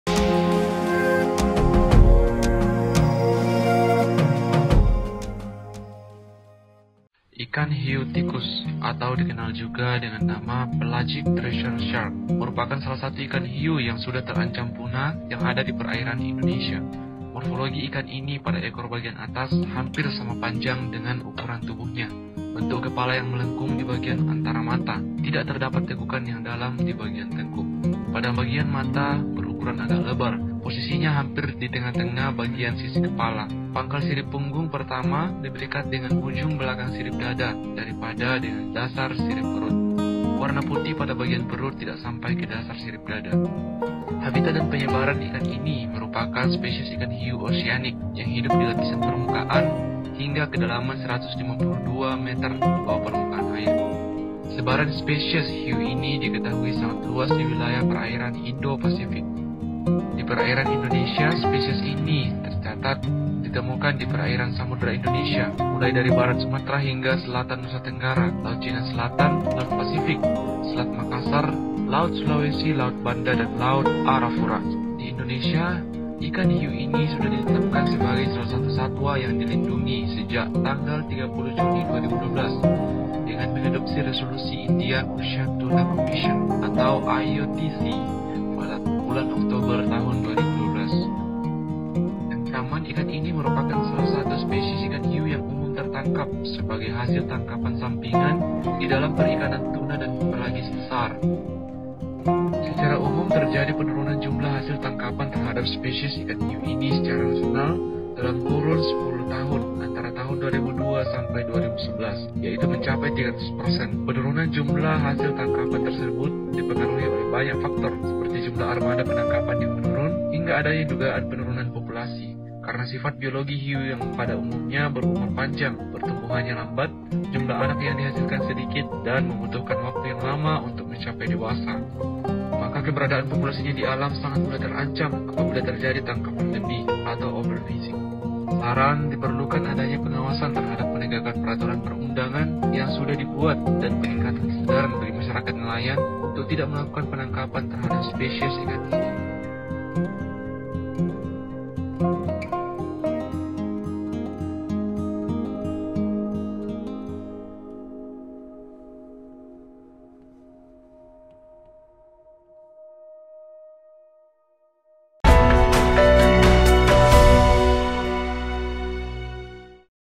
Biota Perairan Indonesia Yang Terancam Punah #2 Hiu Tikus | Pelagic Thresher Shark
Thresher pelagis adalah spesies hiu perontok, keluarga Alopiidae; kelompok hiu ini ditandai dengan lobus atas yang sangat memanjang dari sirip ekor mereka. Thresher pelagis dapat ditemokan di perairan tropis dan subtropis di Samudra Hindia dan Pasifik, biasanya jauh dari pantai, tetapi kadang-kadang memasuki habitat pesisir.

I'm on Instagram as @rex_zeno. Install the app to follow my photos and videos.